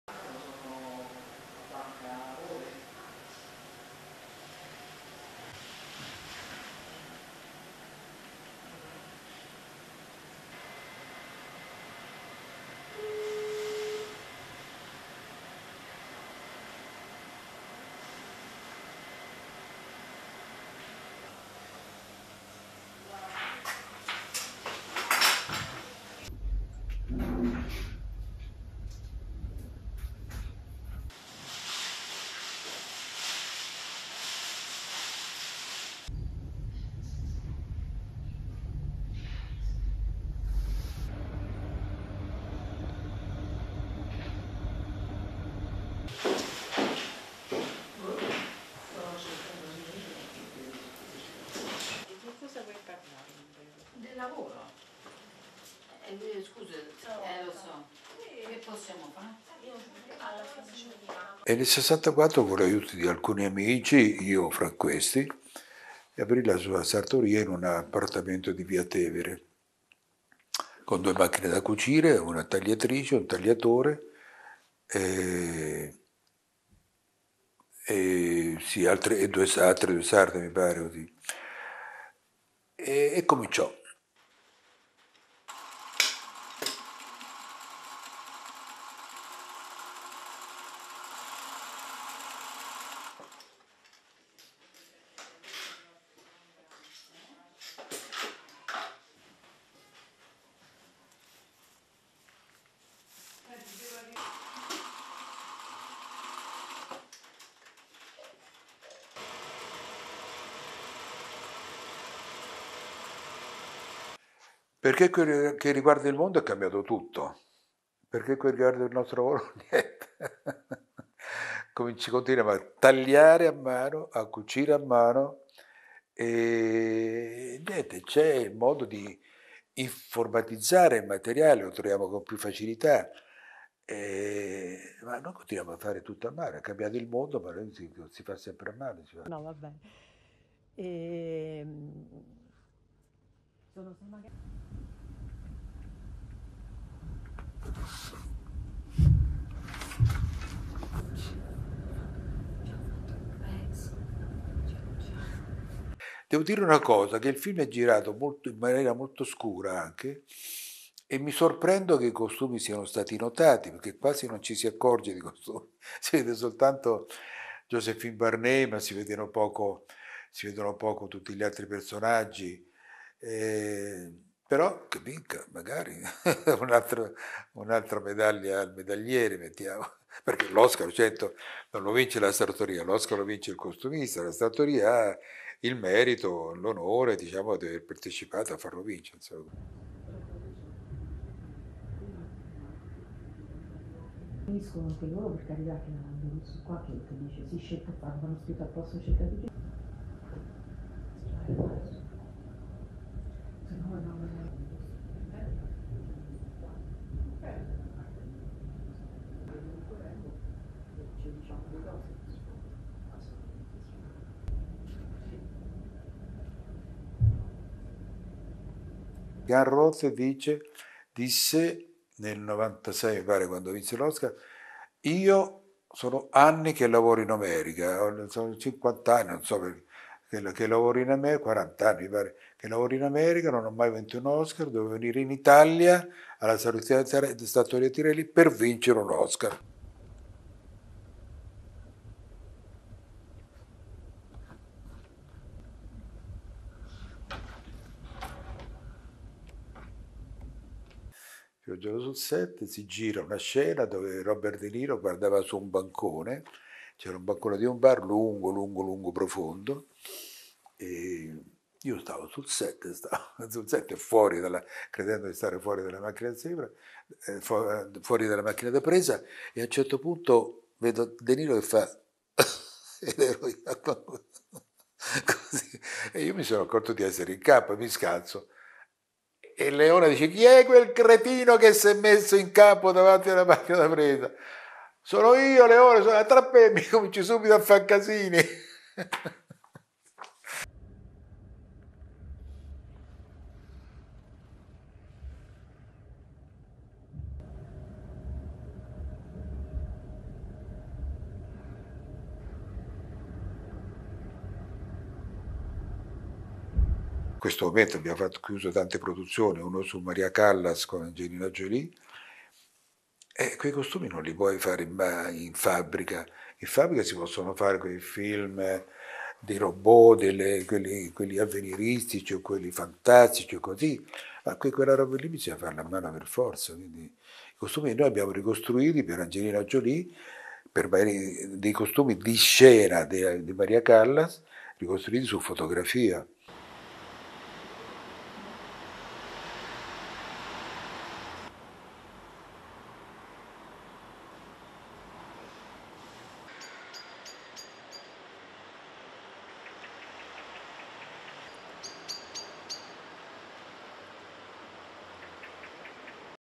A costume atelier in Rome is behind 17 Oscar wins for costume design. Will 'Napoleon' be next?
ITALY OSCARS ATELIER

RESTRICTION SUMMARY:

LENGTH: 5:56

ASSOCIATED PRESS
Rome, Italy -  27 February 2024

1. Wide of dresses displayed in Tirelli tailoring house's atelier (studio) From the left: two costumes designed for Italian TV series “Il commissario Ricciardi” and a white dress designed by costume designers Janty Yates and David Crossman for Marie Louise (played by Anna Mawn) in Ridley Scott’s film “Napoleon”
2. Mid of Marie Louise’s costume in Ridley Scott’s “Napoleon”
3. Tilt down of Theodor Pištěk’s costume for Wolfang Amadeus Mozart (played by Tom Hulce) in the film “Amadeus” by Miloš Forman
4. Close up of lace details on Amadeus’ costume
5. Costume designers’ sketches on the walls of Tirelli's studio
6. Antique mannequins in Tirelli’s studio
7. Antique mannequin portraying a woman in front of the wall with costume designer Piero Tosi's sketches hanging behind
8. Close up of a sketch by Tosi depicting the costumes designed for Pier Paolo Pasolini's movie "Medea"
9. Various of the dressing gown created by Gabriella Pescucci for Countess Ellen Olenska (played by Michelle Pfeiffer) in Martin Scorsese’s “The Age of Innocence”
10. Close up of a picture portraying Umberto Tirelli hugging a mannequin
11. Close up of an old article reading (Italian): “Works for filmmakers all over the world. A collection of around 4000 dresses, from the 18th century to the present day. Cutting and sewing, Oscar-worthy tailoring. The Tirelli atelier made the award-winning costumes for the film Moulin Rouge.”

12. SOUNDBITE (Italian) Dino Trappetti, President of Tirelli tailoring house:
“In 1964, with the help of some friends, me among them, (Umberto Tirelli) opened his tailor's shop in a flat on Via Tevere, with a sewing machine, two cutters and two other seamstresses, I think. And so it began.”

13. Various of a seamstress sewing
14. Mid of sewing tools on the table
15. Various of seamstresses working and sewing
16. SOUNDBITE (Italian) Dino Trappetti, President of Tirelli tailoring house:
"As regards to the world, everything has changed. As for our work, nothing has changed. We continue to cut by hand and sew by hand. There is a way to computerise the material, we find it easier, but we continue to do everything by hand. The world has changed, but we have not.”

ASSOCIATED PRESS
Formello, Italy – 27 February 2024

17. Wide of costumes hung and stored in the 7000 square metre warehouses in Formello near Rome
18. Tilt down of the costume designed by Janty Yates and David Crossman for Hortense (played by actress Isabella Brownson) in Ridley Scott’s movie “Napoleon”
19. Mid of the costume designed by Janty Yates and David Crossman for Emperor Francis I (played by actor Miles Jupp) in Ridley Scott’s movie “Napoleon”

20. SOUNDBITE (Italian) Dino Trappetti, President of Tirelli tailoring house:
“I must say one thing, that the film is shot very darkly, and I am surprised that the costumes were noticed, because you hardly notice the costumes. You only see Joséphine de Beauharnais, you hardly see all the other characters. But maybe it will win! Let's add another medal to the medal collection. Of course, the Oscar is not won by the tailor's shop, the Oscar is won by the costume designer, but the tailor's shop has the merit and the honour of having participated to make it win.”

22. Various of dresses displayed in the warehouse

ASSOCIATED PRESS

ASSOCIATED PRESS